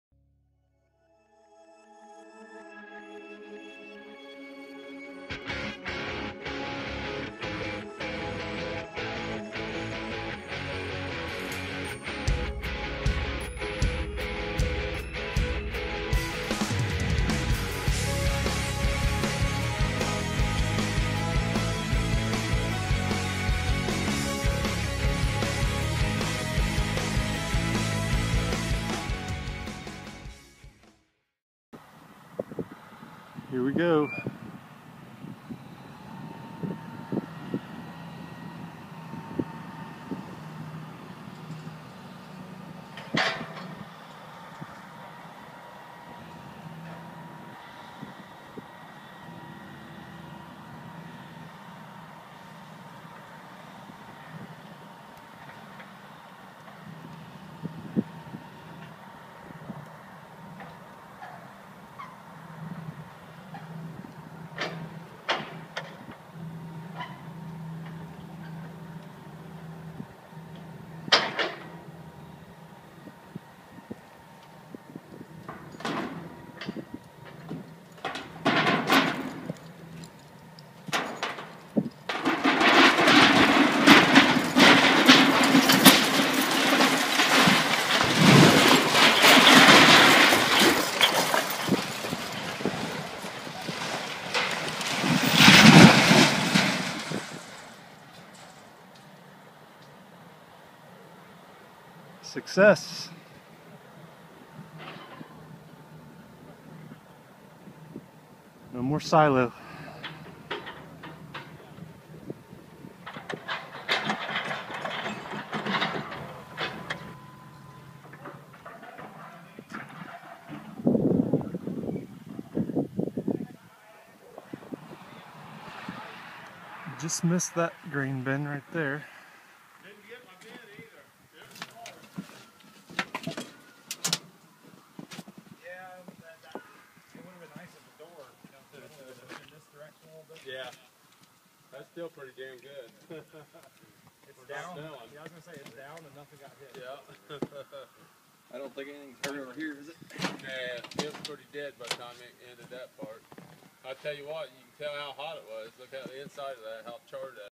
Silo Demolition Success
Successful attempt at knocking down an old concrete silo. Just barely missed the barn and grain bin.